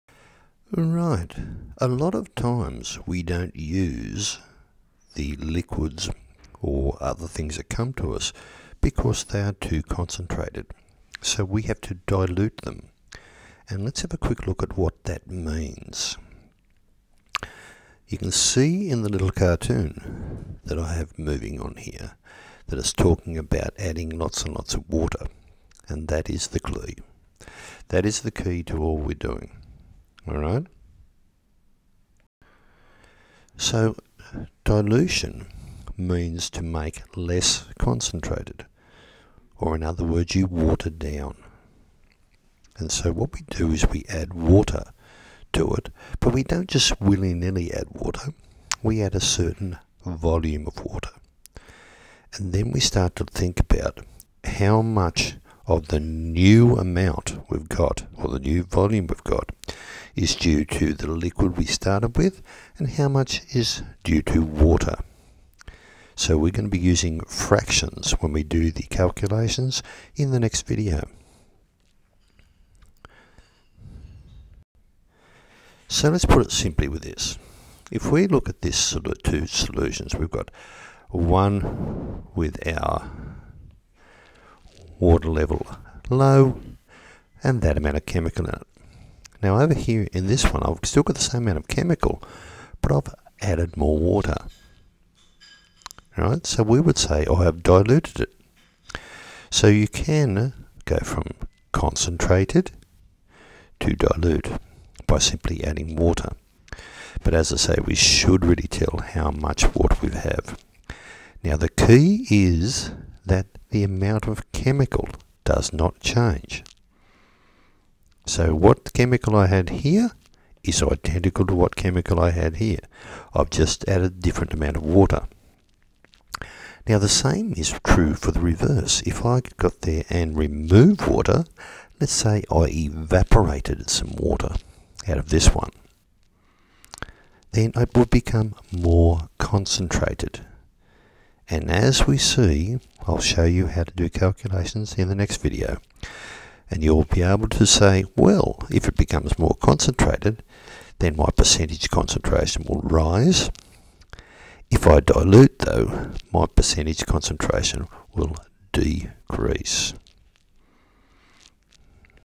Dilutions 1
Relating dilutions to adding water
The amount of chemical is unchanged and that is an important fact to remember
If water is added our concentration;gets smaller, but if water is removed by evaporation then our concentration increases

00:00 Slide 1
00:32 Slide 2
01:15 Slide 3